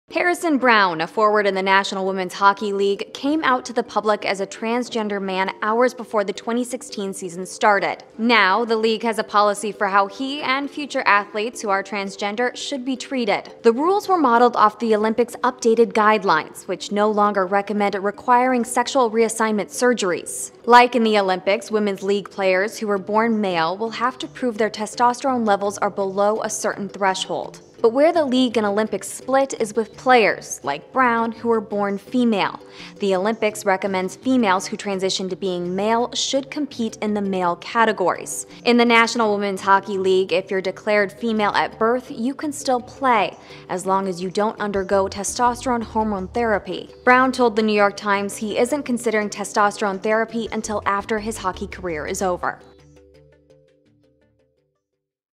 NWHL releases transgender policy
The National Women's Hockey League is the only professional sports league in North America with a player who is openly transgender.